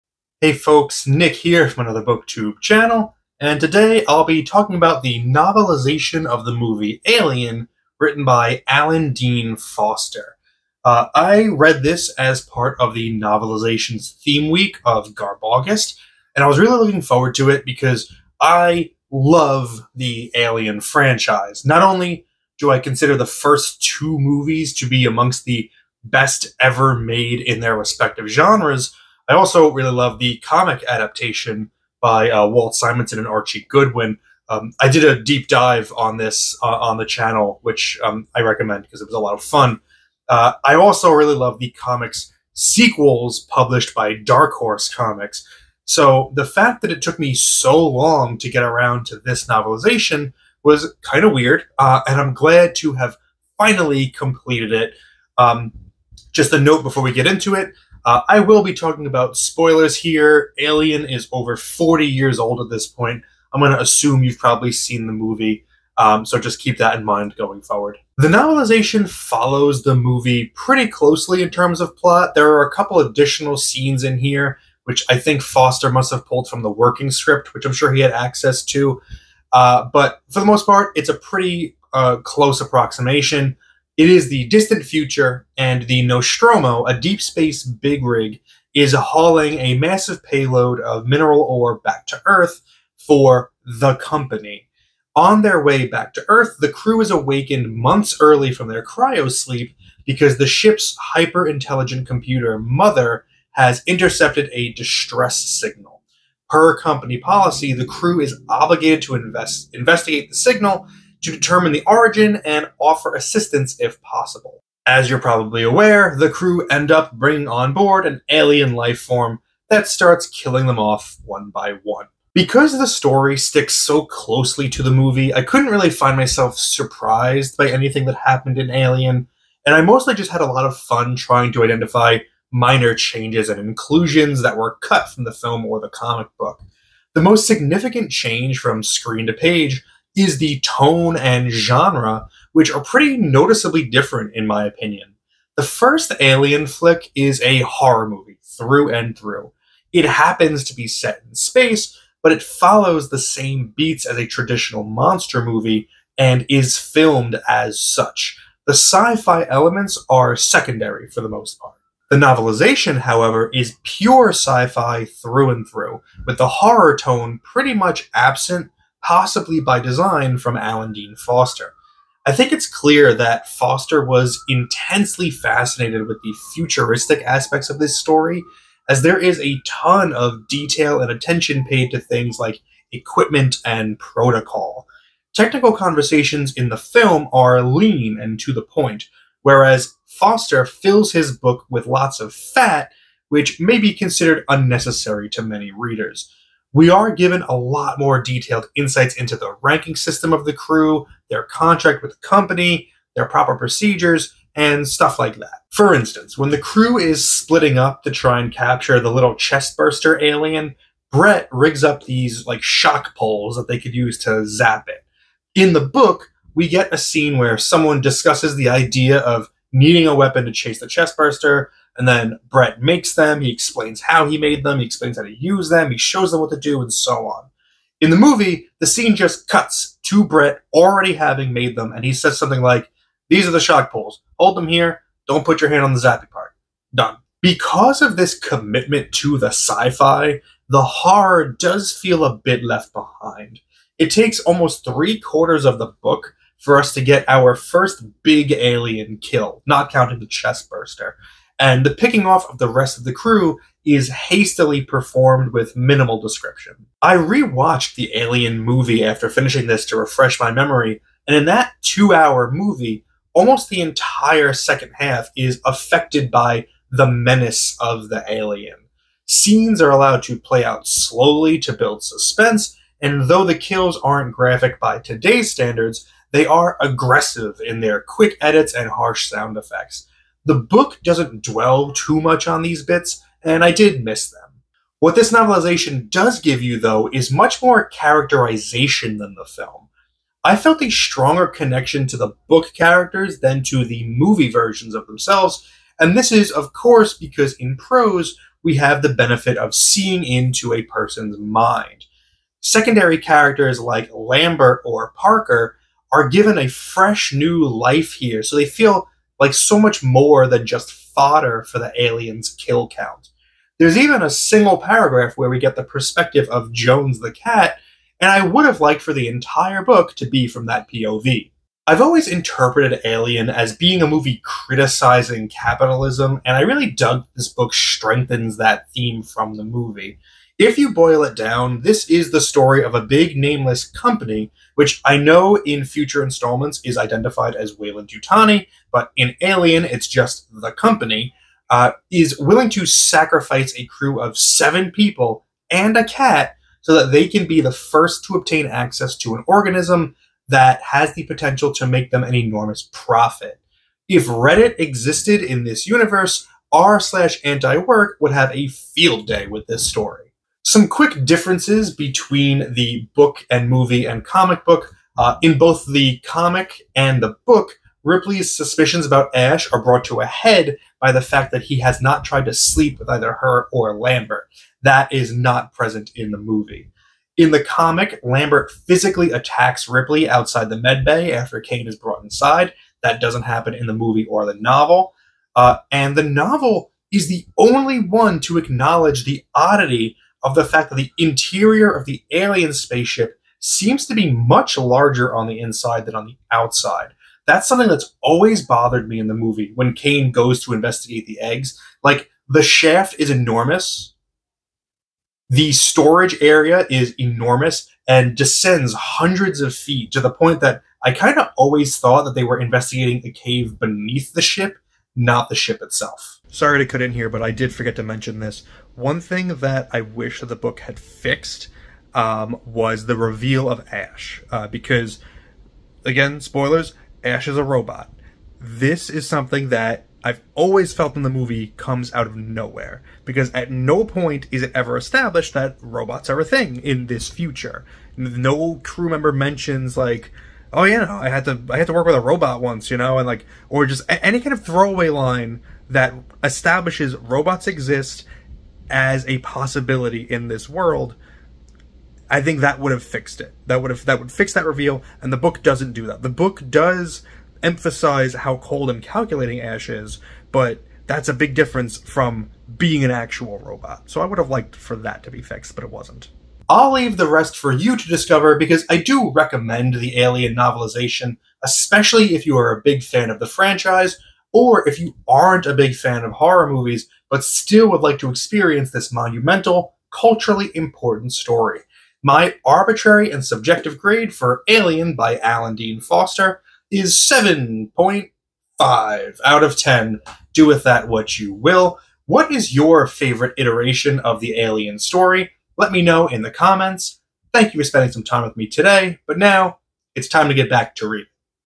Alien - Novelization of the Film by Alan Dean Foster | Book Review
I read this as part of the Novelizations theme week of GarbAugust.

The Weeks:
Week 1 - Category Romance or Men's Adventure
Week 2 - Paperbacks from Hell or Vintage Smut
Week 3 - Novelizations or Books by Famous People
Week 4 - Anything Goes...anything [winks]